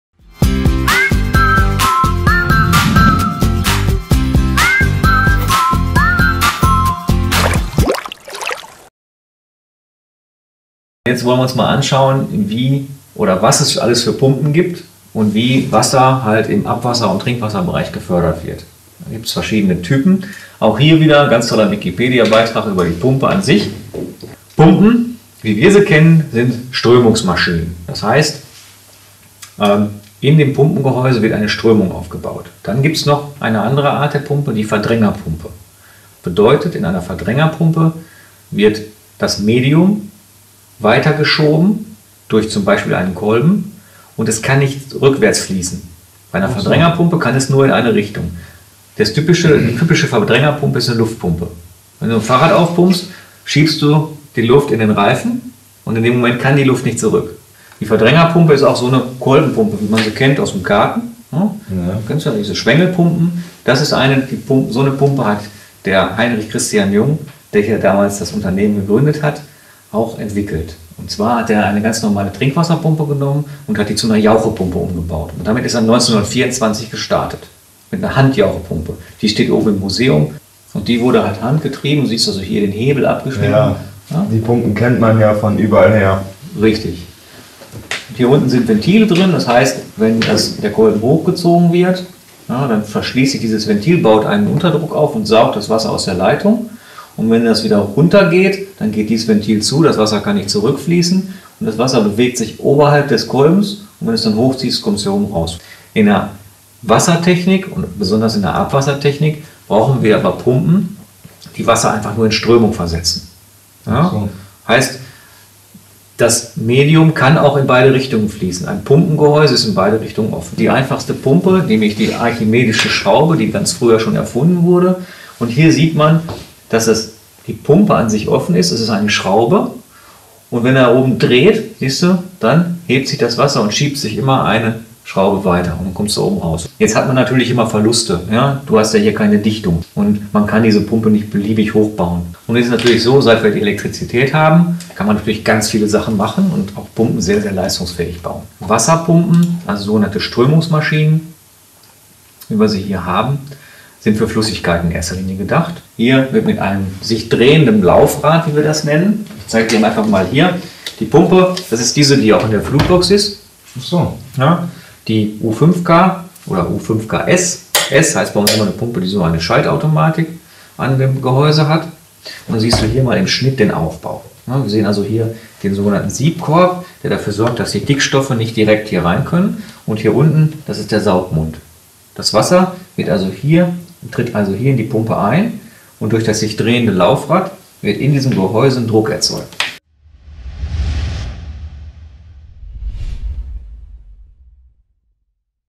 Dwight fragt Jung Pumpen antwortet [5] - Wie funktionieren Pumpen (Teil 1)
In diesem Video erklären wir Dwight, welche Arten von Pumpen es überhaupt gibt und wie sie sich in der Funktionalität unterscheiden.

Jens Höwener unser technischer Trainer klärt Dwight auf.